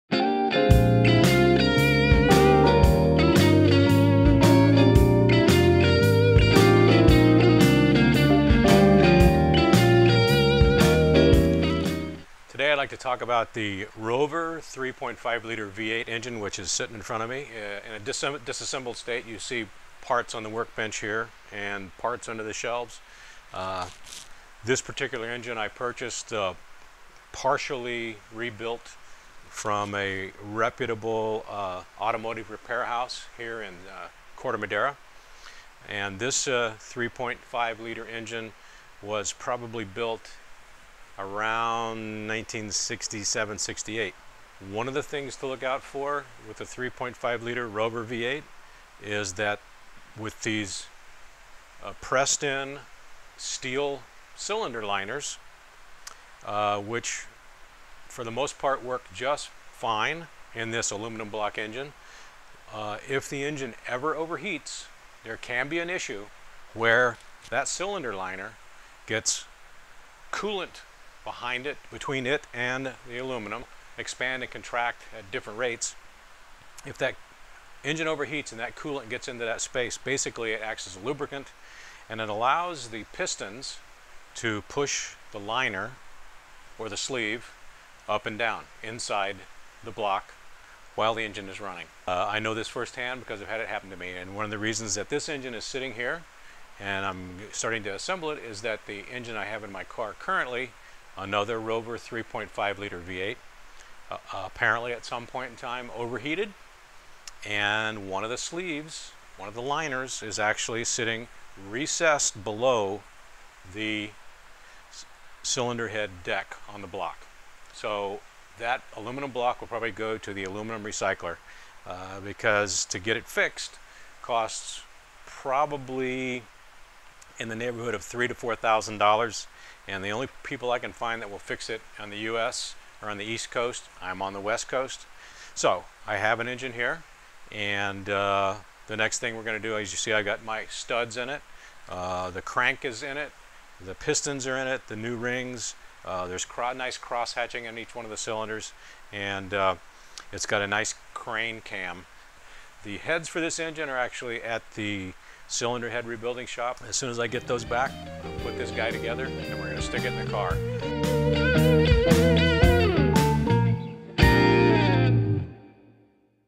Rover V8 Episode 1 - The "Slipped Liner"v2 .mov
Ken Smith of Kenzmyth Productions presents an explanation of the slipped cylinder liner on his  3.5L Rover V8 conversion in a 1966 MGB.